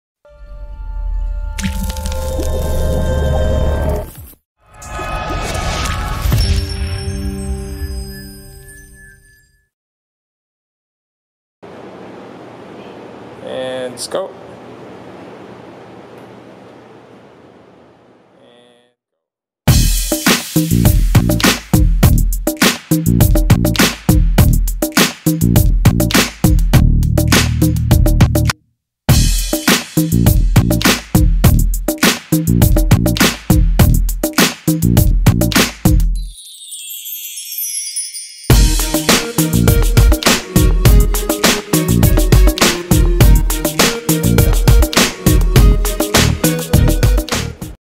Hanging Leg Raise
Hanging leg raises primarily works the hip flexor muscles and your hand grip. Initiate by drawing in the stomach and pull the knees towards your chest.

Training equipment used in the video:
Cable Machine - Inspire Fitness FT2 Functional Trainer

Location: Chris Sports Experience Center